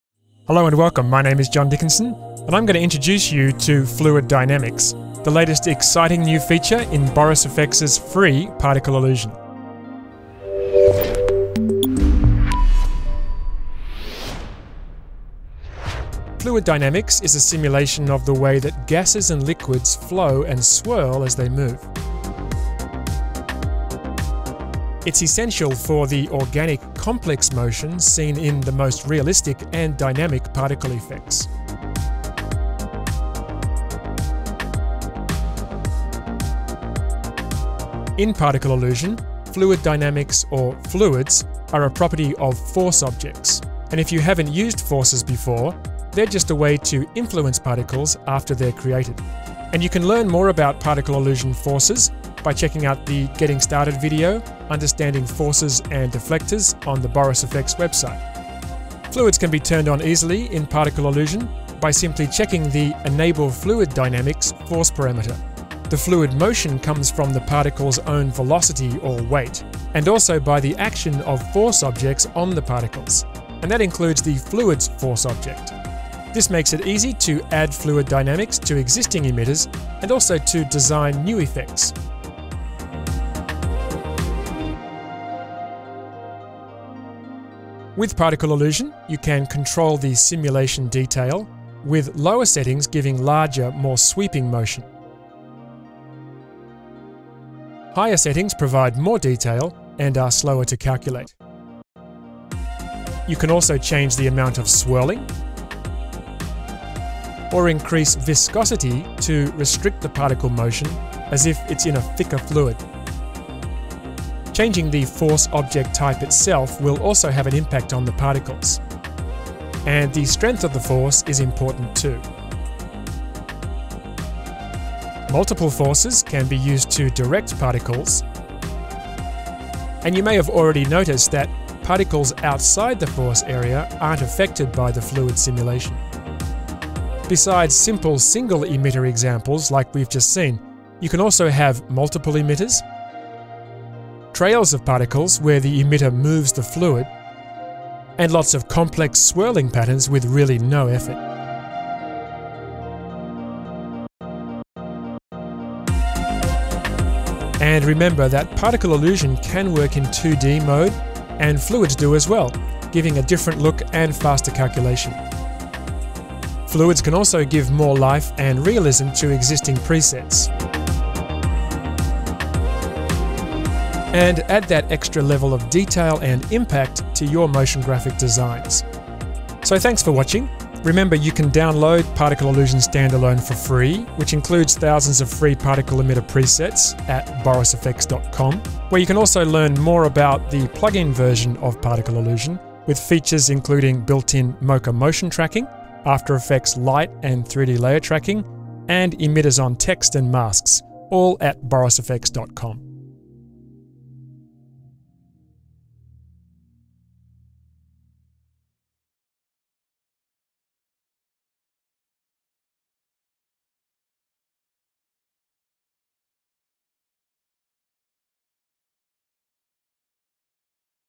Boris FX Particle Illusion 2022 - New Fluid Dynamics
Join John Dickinson as he introduces the new Particle Illusion 2022 standalone application updates. John will break down the new Fluid Dynamics feature and demonstrate how it can enhance your Particle Illusion projects.